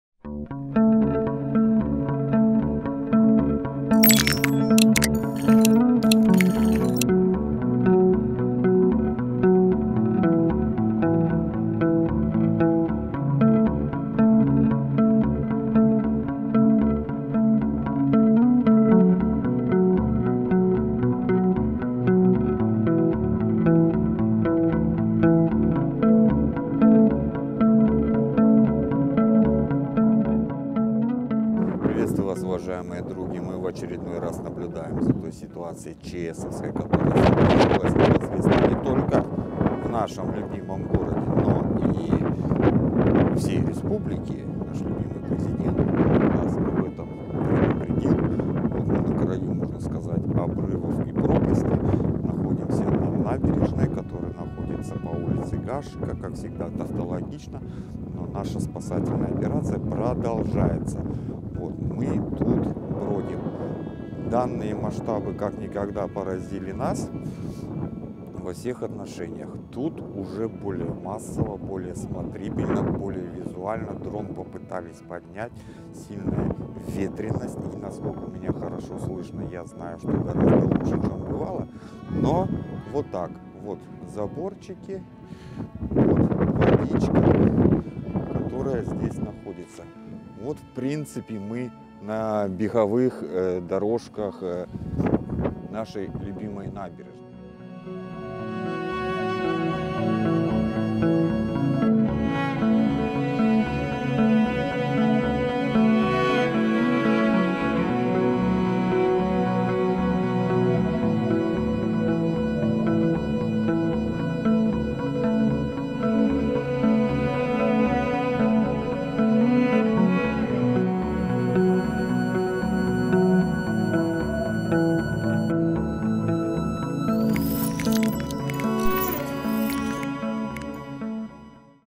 Набережная 25 лития независимости Республики Казахстан. Ситуация ЧС
Мы следим за ЧС ситуацией на водоемах города Костанай. Набережная 25 лития независимости Республики Казахстан. Дата сьемки вечер 6 апреля. Ситуация быстротечно меняется.